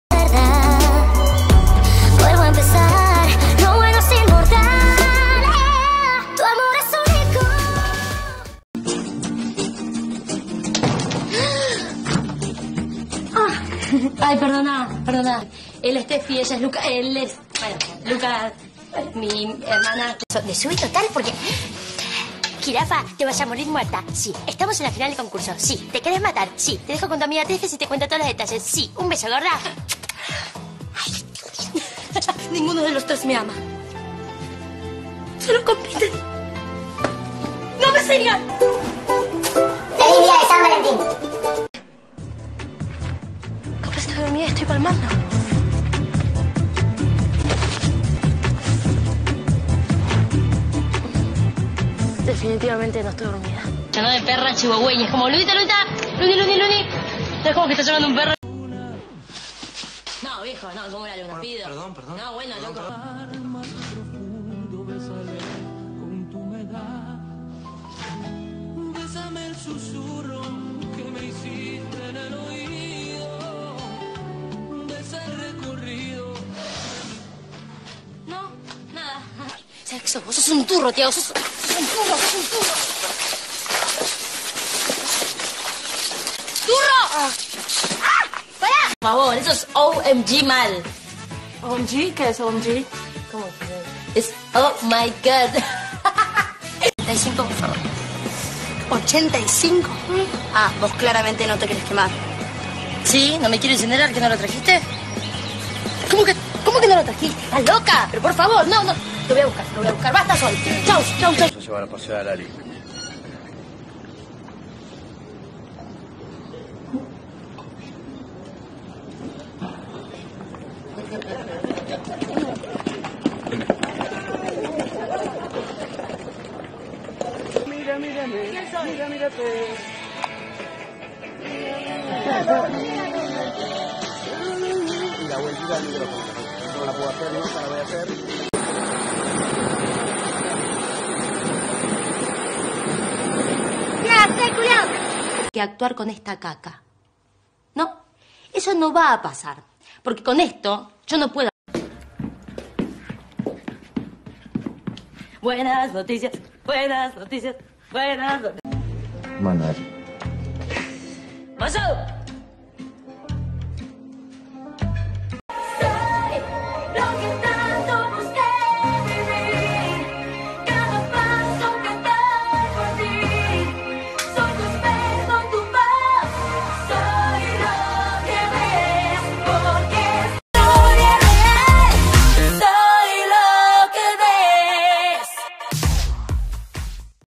Especial Momentos divertidos de Lali Esposito
Aquí os dejo un poco de algunos momentos de Lali tanto actuando , como no , tiene muchos pero os dejo unos pocos , espero que os gusten estos vídeos así subir más :)